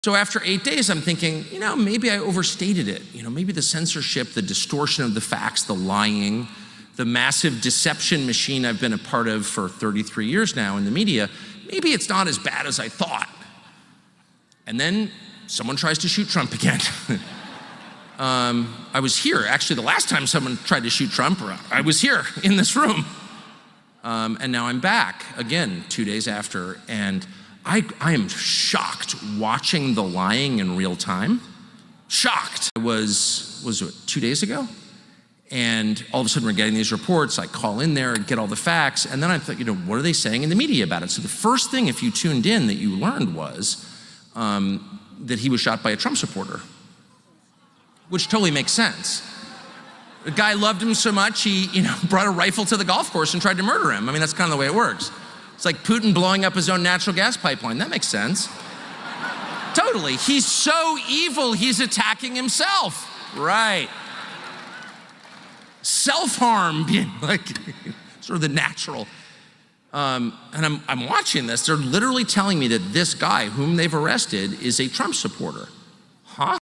Shocking Media Lies 😲 About Trump's Near-Death Experience! 🎯
Join us as we dive into the explosive discussion on media manipulation and the twisted narratives surrounding the recent attempted attack on Trump. Prepare for eye-opening revelations you won’t want to miss!